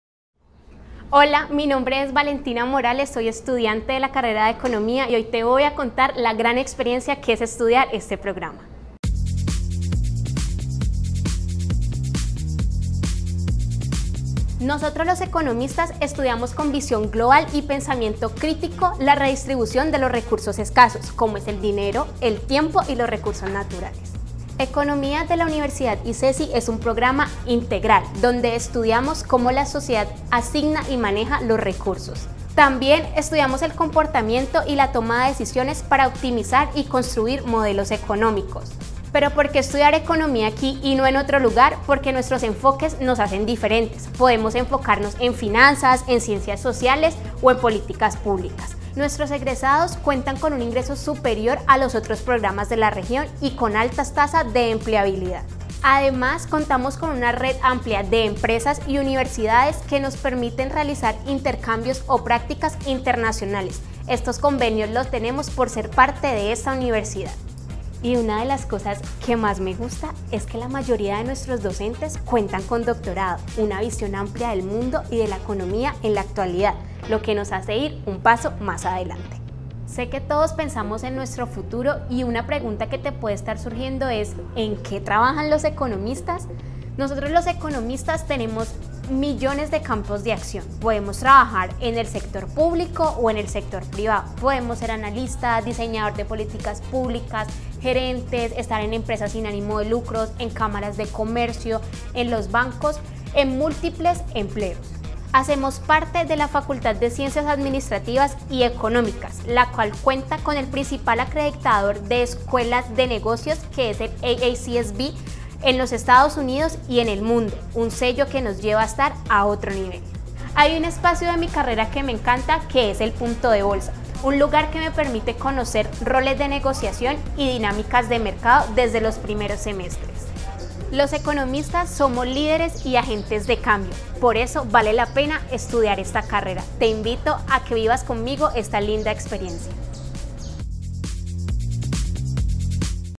Economía
Si te interesan los asuntos económicos y sociales; prepárate para una carrera donde le puedes sacar partido a tu talento, destacándote en estos asuntos, con perspectivas amplias para trabajar por el progreso, a través del liderazgo en posiciones de avanzada en el sector privado, el gobierno y en organismos multilaterales.